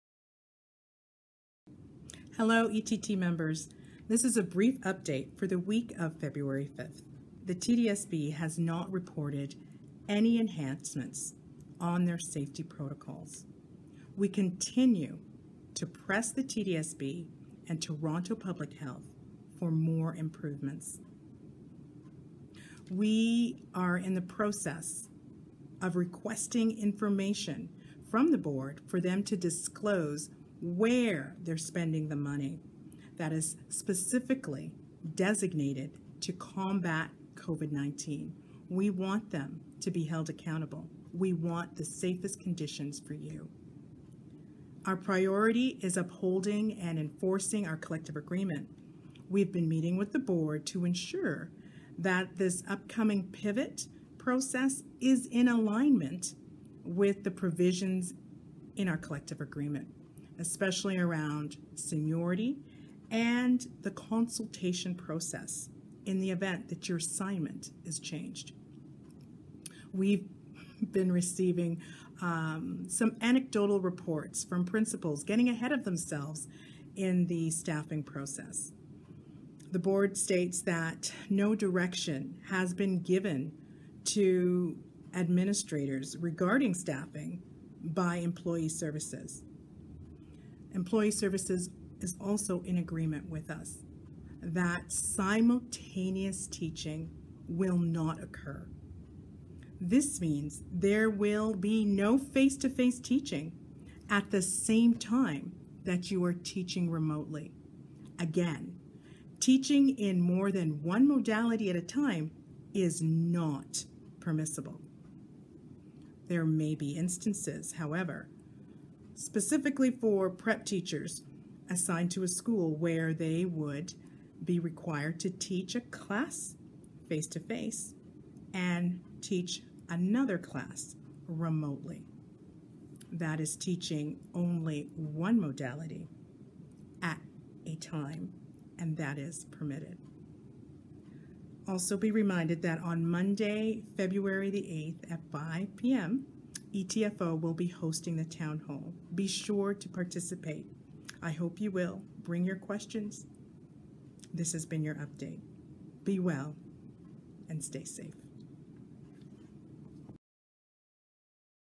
Video Update From ETT President Jennifer Brown - February 5, 2021
In her video address for the week of February 5, 2021, ETT President Jennifer Brown discusses the pending return to in-person learning, the Board's February pivot, staffing, simultaneous teaching, and ETFO's upcoming telephone town hall on February 8.